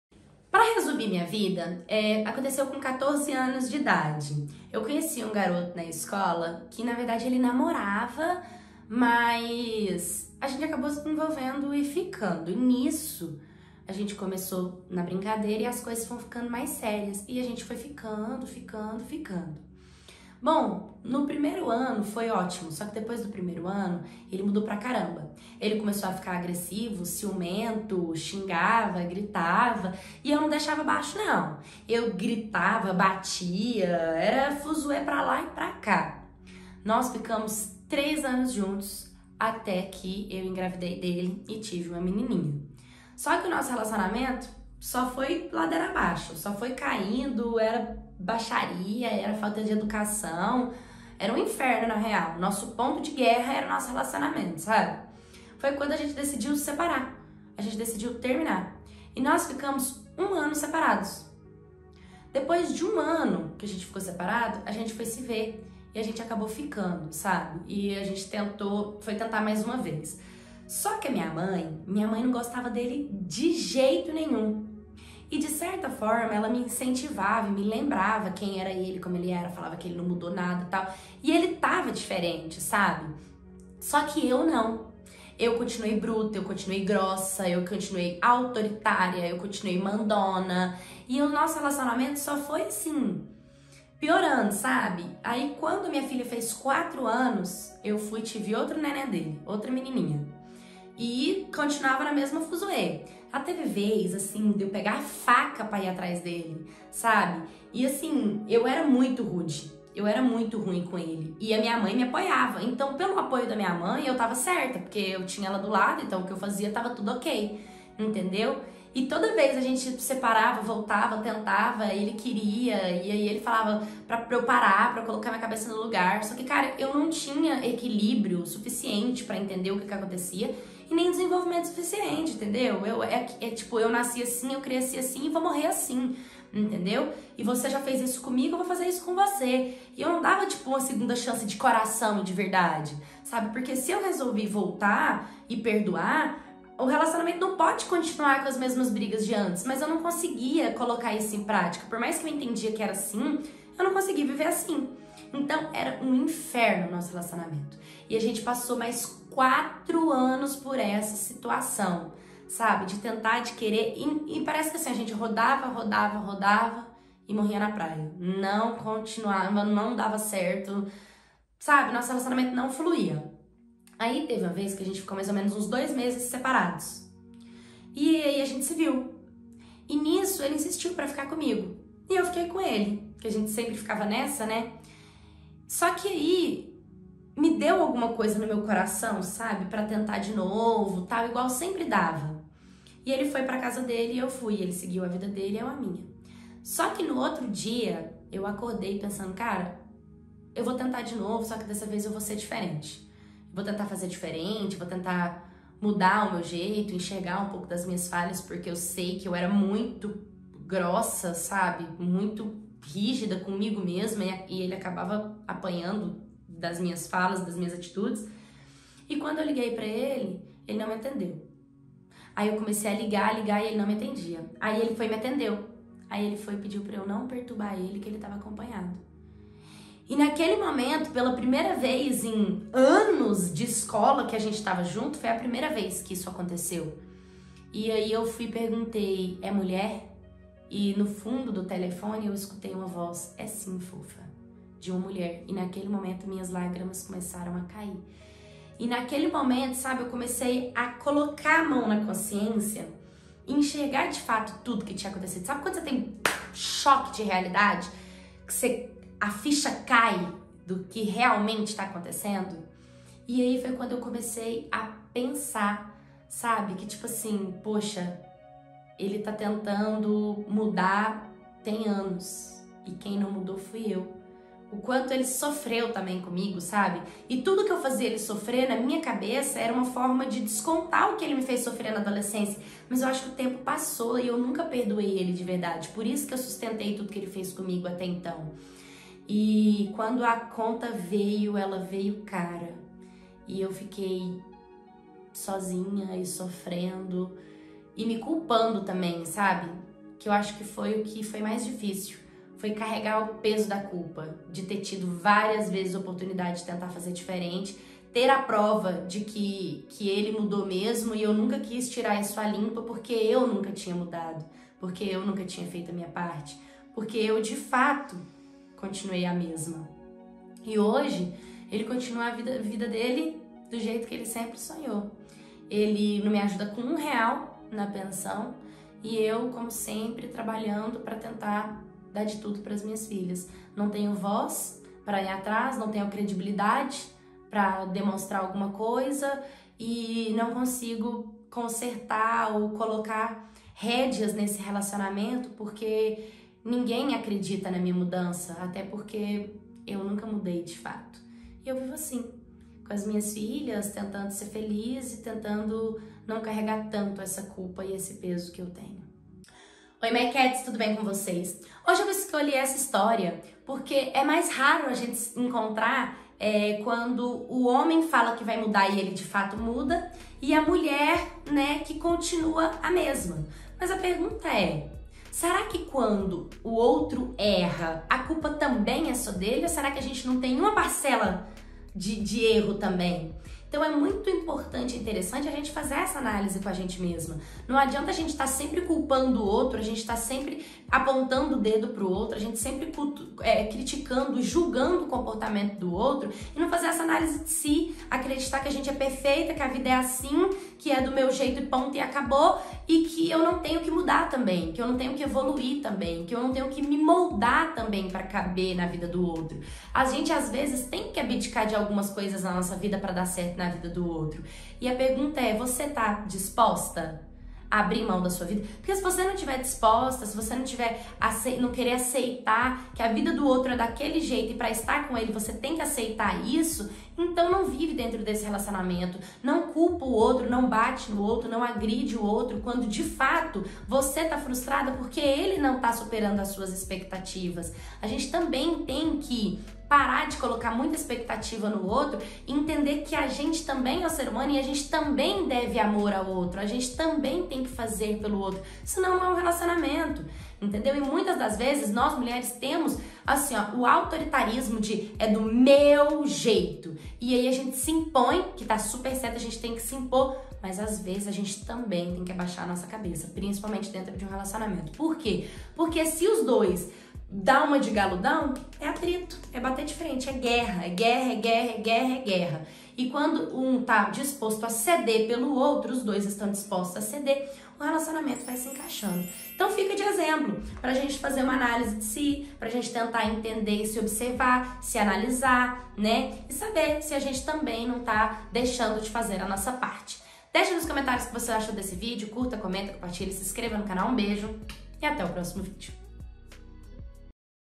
COLEGA DA ESCOLA| DIA DE PAULA
No vídeo de hoje eu conto a história de uma seguidora que começou um namoro de escola no entanto sua mãe não gostava dele de jeito nenhum, e por causa desse namoro os dois mudaram completamente pra pior, o rapaz conseguiu mudar já a menina...